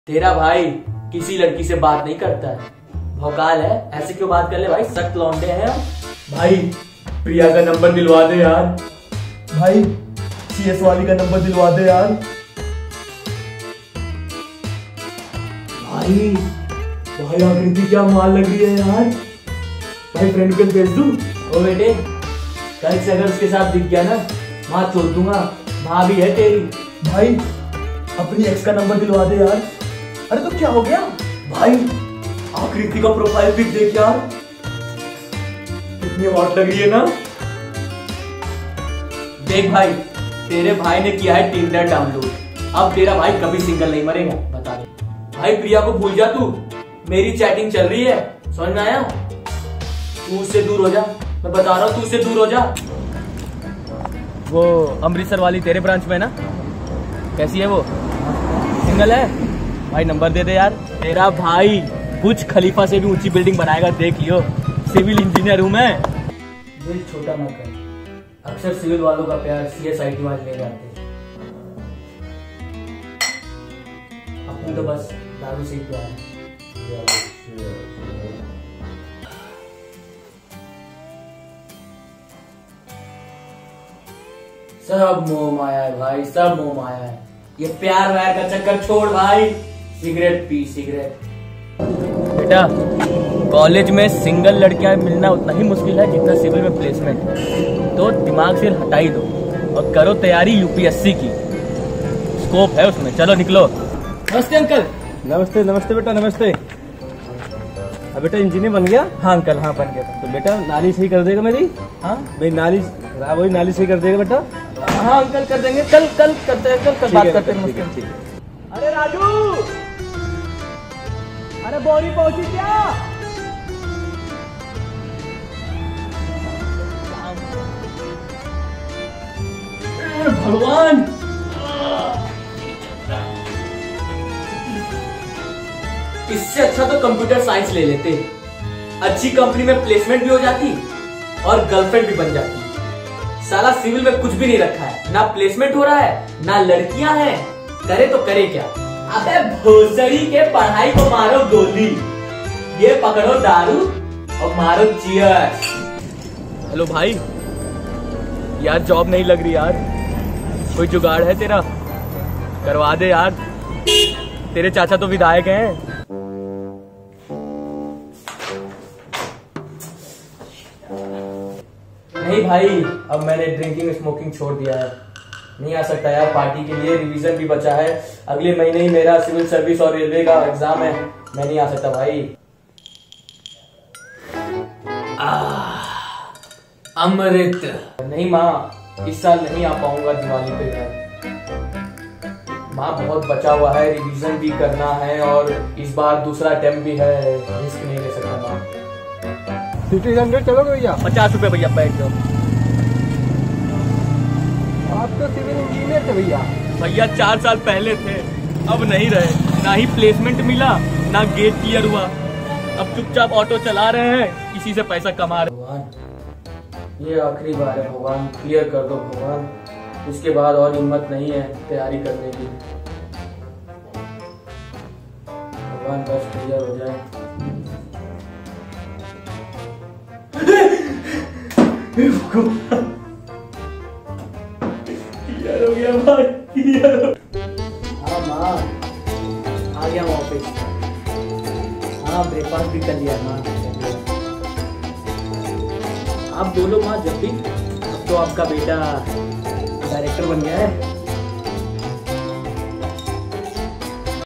Story of Every Civil Engineer |Life of Engineer | Ft.Abhishek Singh
Hey guys! Check out this video and let us know what you think about it in the comments below!

Credits-
Written and Directed by Abhishek Singh
DOP and Edited by Aadeesh Jain

Cast: Abhishek Singh, Nikhil, Ankit, Hemant, Prem Ankit Singh